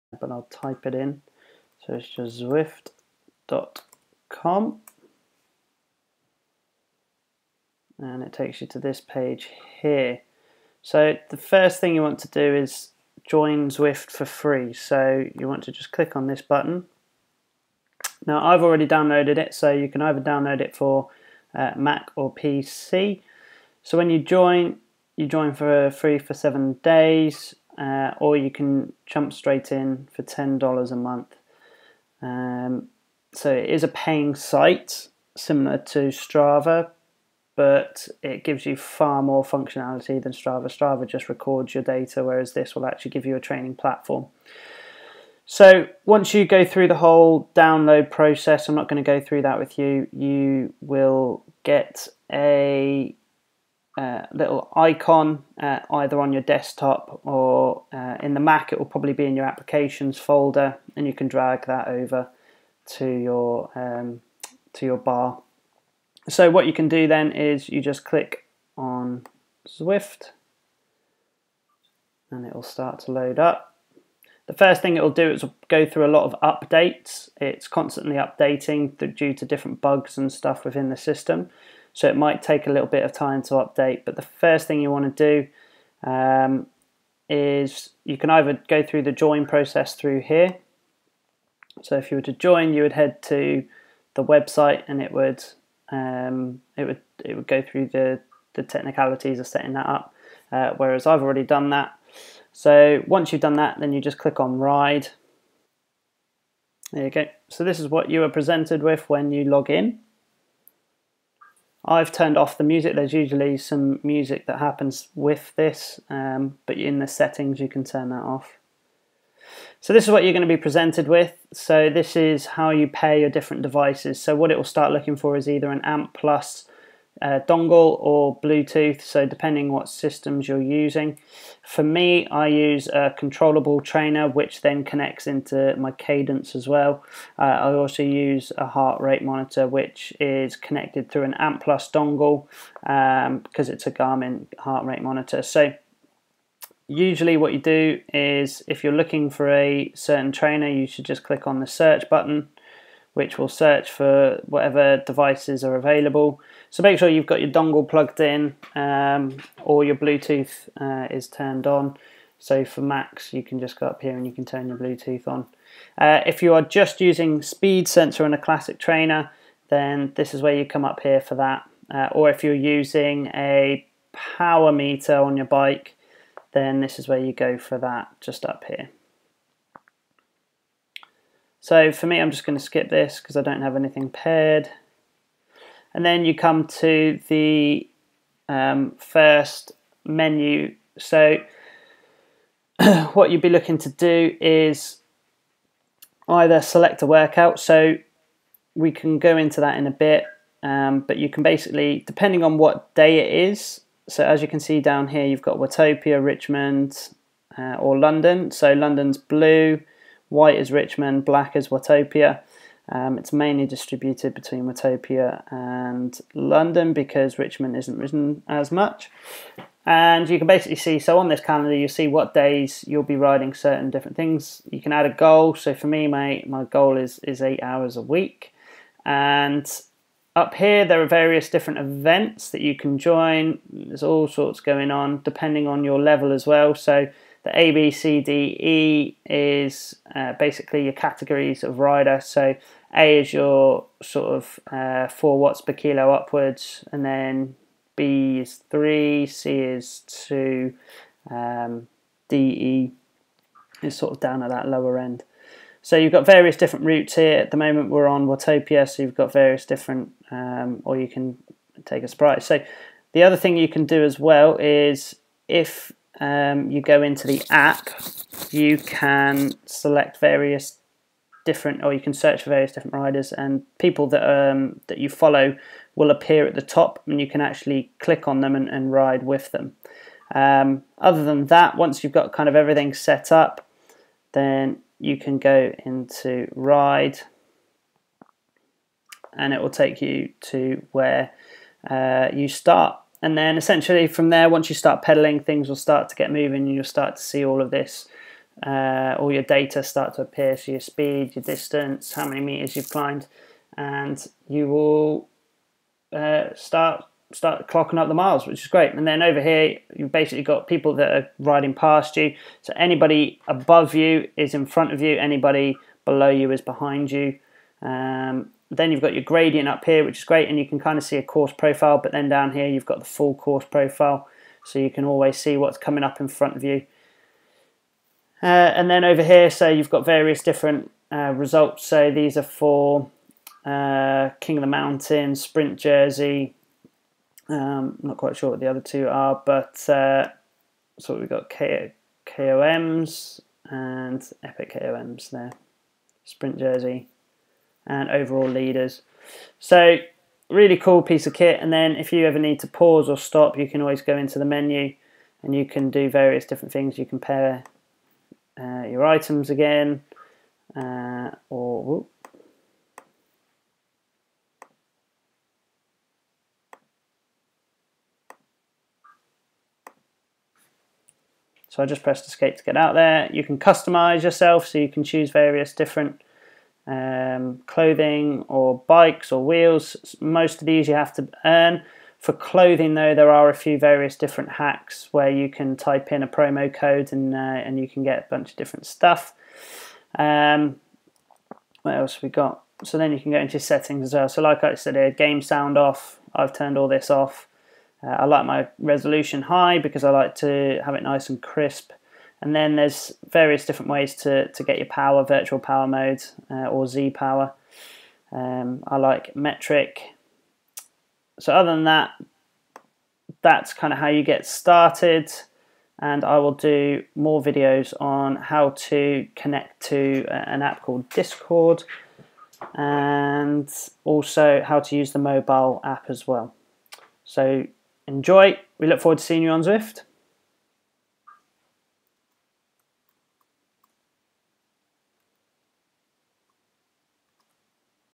How to set up Zwift on your pc or mac
This is a quick video on how to get started with zwift. There are lots of blogs out there but this is a video tutorial running you through step by step on how to easily set up zwift on your pc or mac.